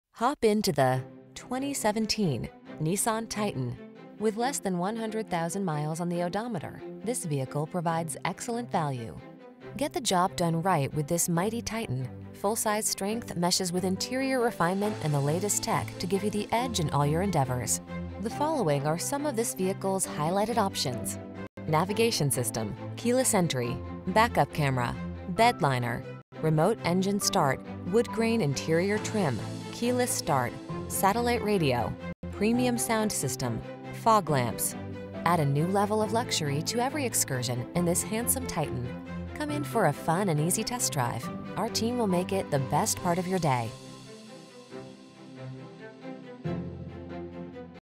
2017 Nissan Titan Carrollton, Abilene, Fairplay, Whiteburg, Mt. Zion GA P5832
Gun Metallic Used 2017 Nissan Titan SL available in 725 Bankhead HWY, Carroliton, Georgia at Scott Evans Nissan. Servicing the Carroliton, Abilene, Fairplay, Whiteburg, Mt. Zion, GA area.